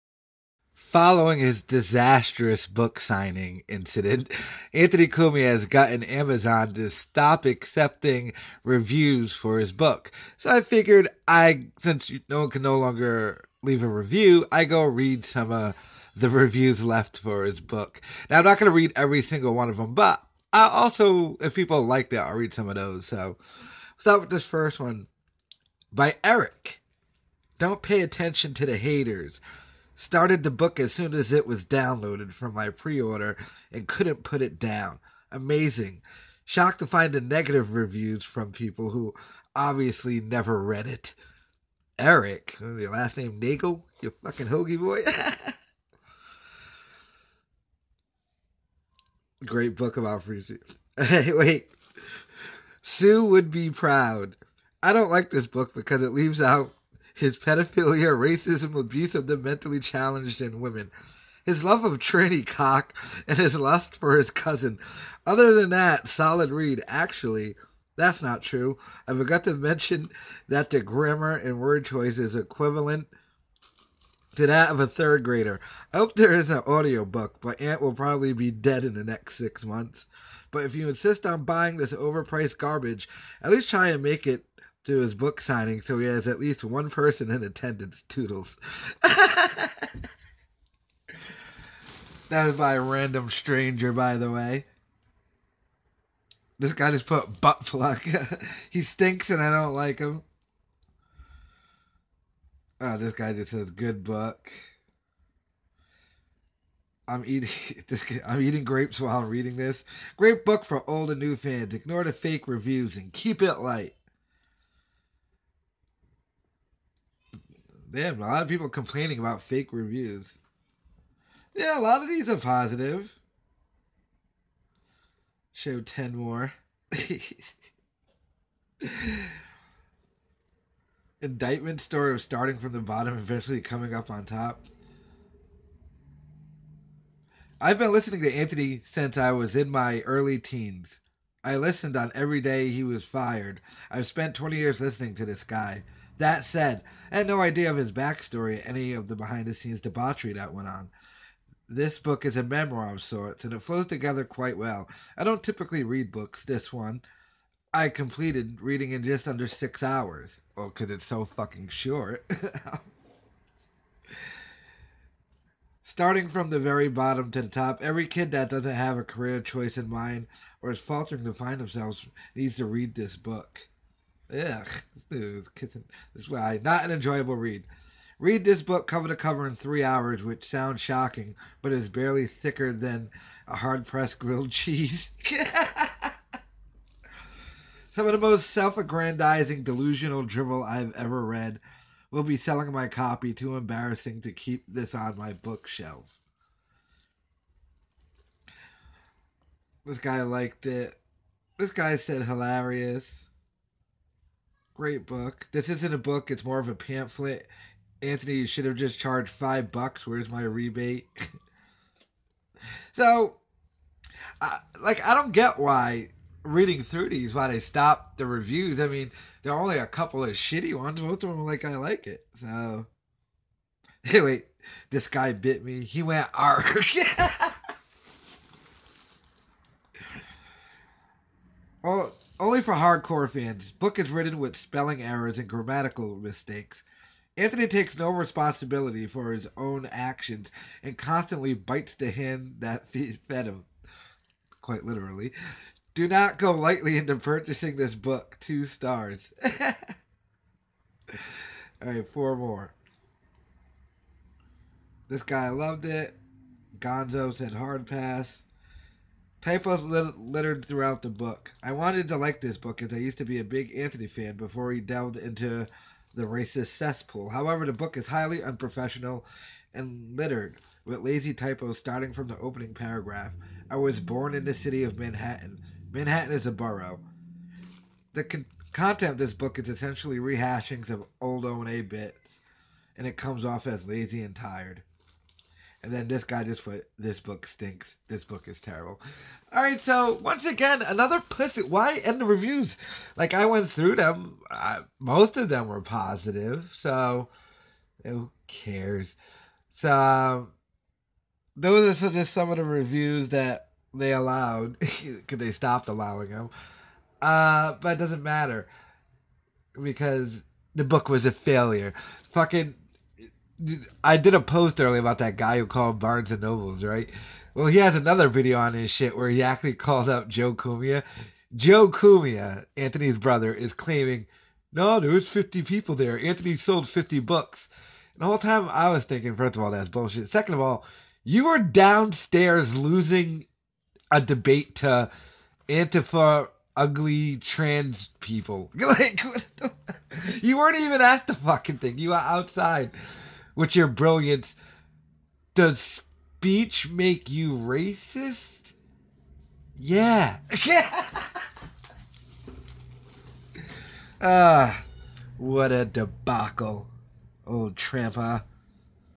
Reading Reviews Of Anthony Cumias Book Permanently Suspended!!!
Reading Amazon reviews of the former Opie and Anthony show co host who was fired by Sirius XM radio book